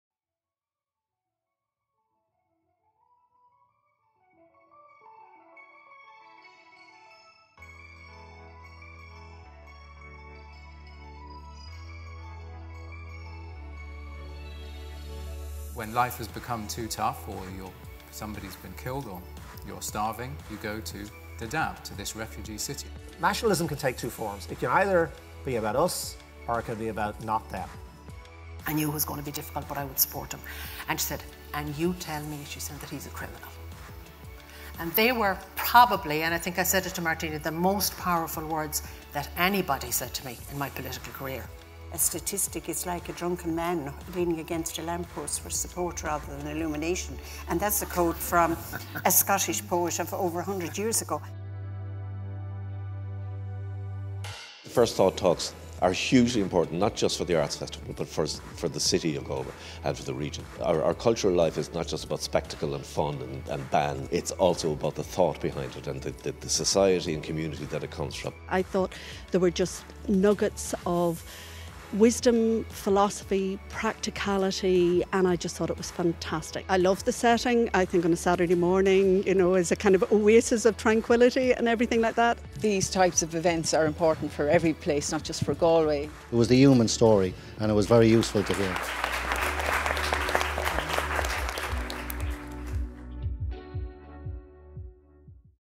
Highlights from GIAF18 Winter First Thought Talks
Early this winter (30 November -- 1 December 2018), Galway International Arts Festival held its inaugural Winter First Thought Talks speaker series in the Aula Maxima, NUI Galway. Watch this video to hear what audiences had to say, and hear excerpts (Full recordings of the four Talks are available to view

The high profile speaker line-up included City of Thorns author Ben Rawlence, Irish Times columnist Fintan O’Toole, Madam Politician author Martina Fitzgerald, former politician Máire Geoghegan-Quinn Labour Party politician Jan O'Sullivan, and tireless advocate for the homeless Alice Leahy Alice Leahy, founder of Trust (now the Alice Leahy Trust).

First Thought Talks have long been a popular part of the summer Festival and the Festival 2018 programme was opened by President Michael D. Higgins. However, this programme marked the first Winter programme and we are delighted to say it was a success.

Don't want to miss a beat on Galway International Arts Festival 2019?